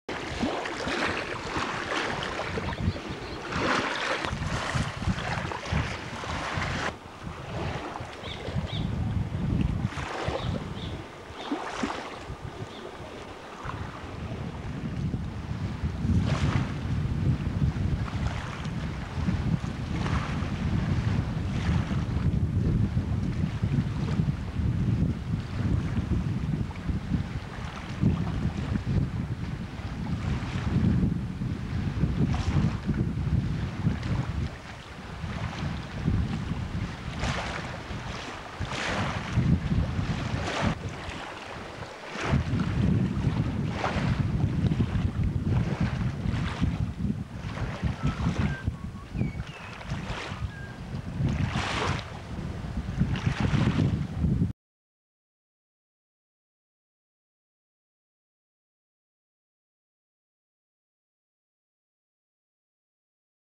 Goanna, Lace Monitor Lizard eating a fish, Pamamaroo Lake, NSW Outback Australia.
This Goanna, the Australian Lace monitor lizard, was filmed at Pamamaroo Lake, Outback Australia.